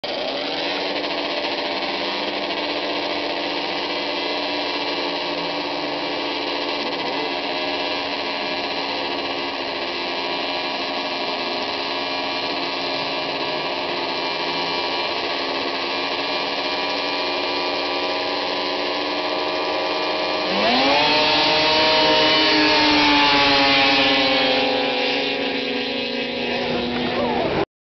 Vintage snowmobile grass drags
One of the grass drags north of duluth,mn. sept.2010